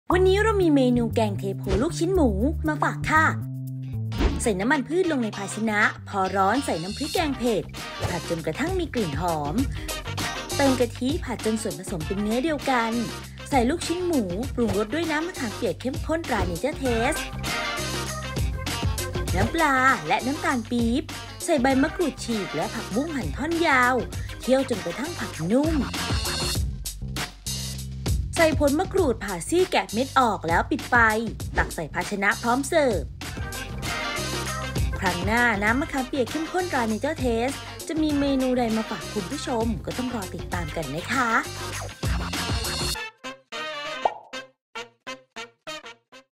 แกงเทโพลูกชิ้นหมู สูตรเด็ด ทำง่าย สไตล์ "อ.ยิ่งศักดิ์"
แกงเทโพลูกชิ้นหมู
Thai Northeastern Style Jaew Shrimp

ผลิตภัณฑ์:น้ำมะขามเปียกเข้มข้น ตราเนเจอร์เทส

น้ำพริกแกงเผ็ด   50 กรัม
กะทิ    250 มิลลิลิตร
ลูกชิ้นหมู   200 กรัม
น้ำมะขามเปียกเข้มข้น ตาเนเจอร์เทส 50 มิลลิลิตร
น้ำปลา    40 มิลลิลิตร
ผักบุ้งหั่นท่อนยาว  500 กรัม
ผลมะกรูดผ่าซีกแกะเม็ดออก 1 ผล

1. ใส่น้ำมันพืชลงในภาชนะ พอร้อนใส่น้ำพริกแกงเผ็ด ผัดจนกระทั่งมีกลิ่นหอม เติมกะทิ ผัดจนส่วนผสมเป็นเนื้อเดียวกัน
2. ใส่ลูกชิ้นหมู ปรุงรสด้วยน้ำมะขามเปียกเข้มข้น ตราเนเจอร์เทส น้ำปลาและน้ำตาลปิ๊บ ใส่ใบมะกรูดฉีกและผักบุ้งหั่นท่อนยาว เคี่ยวจนกระทั่งผักนุ่ม ใส่ผลมะกรูดผ่าซีกแกะเม็ดออก ปิดไฟ
3. ตักใส่ภาชนะ พร้อมเสิร์ฟ

▲Line @yingsakfood